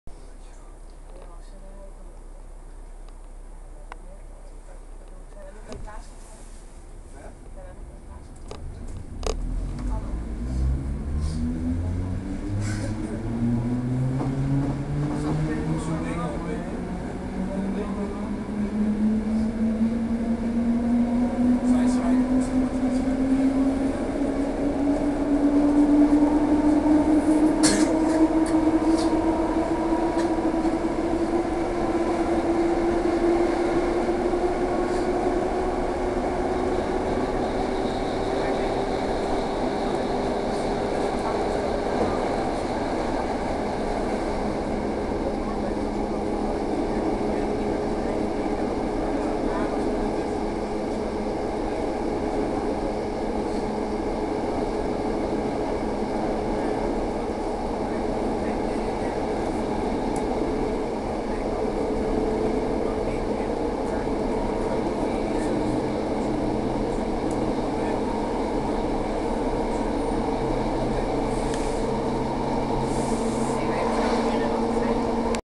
NS Plan-V 905 rijdt met volle vaart station Rijswijk uit!
NS Plan-V rijdt met volle vaart en schitterend geluid station Rijswijk uit. Sorry voor de zon, maar het geluid blijft prachtig.

10-01-'09